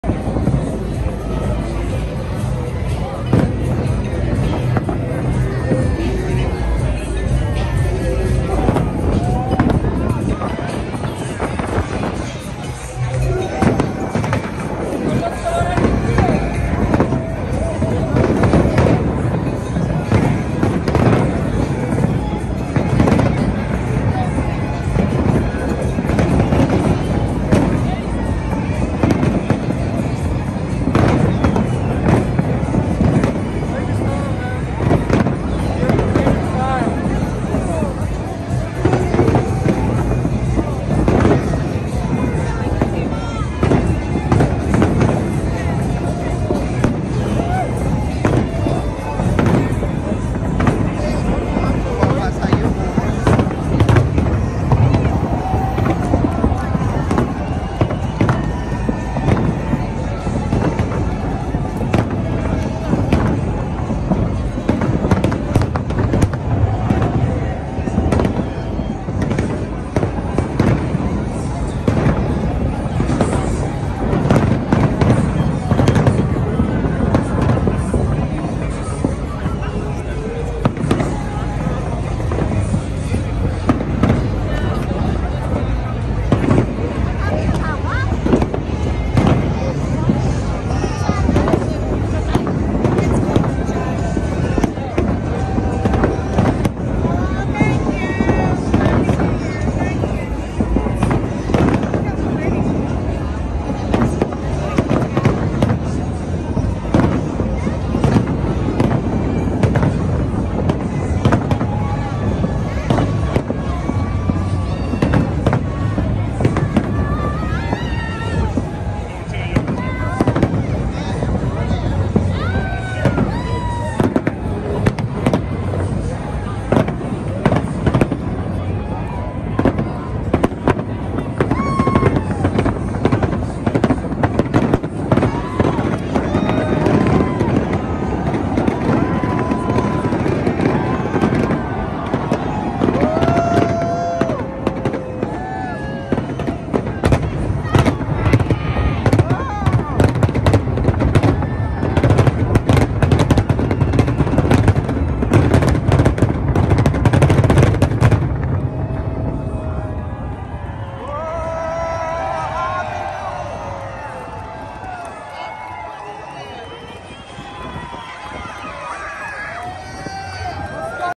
New Year's Eve fireworks display, Las Vegas
New Year's Eve Celebration and fireworks display at The Strip, Las Vegas, Nevada, USA